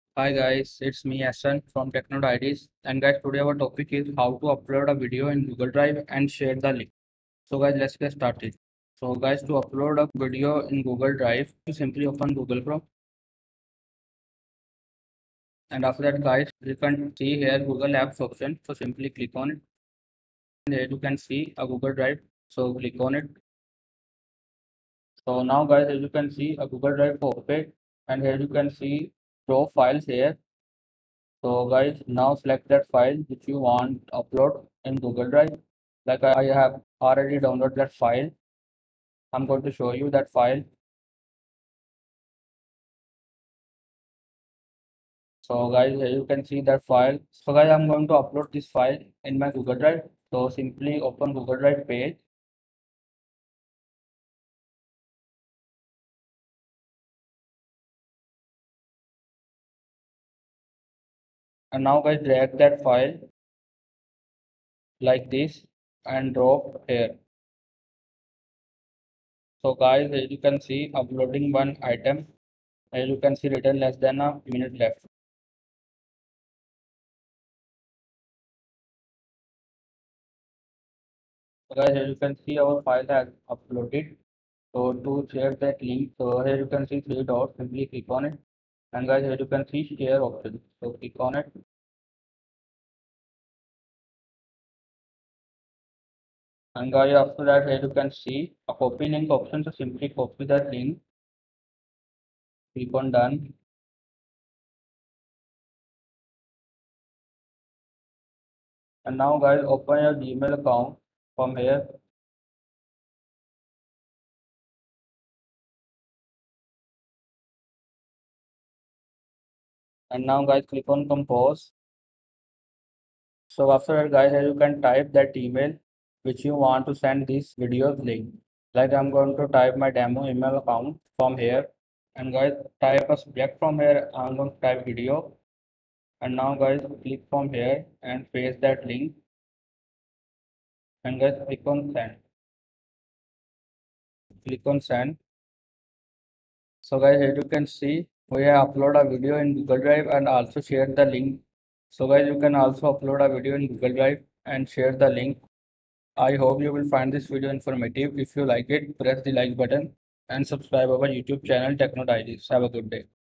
How to upload a video to google drive and share the link (Easy Method)
How to upload a video to google drive and share the link

In this video, i will show you How to upload a video to google drive and share the link. There are very easy steps. I hope it will be helpful to you. Keep watching this video to fix the problem about How to upload a video to google drive and share the link.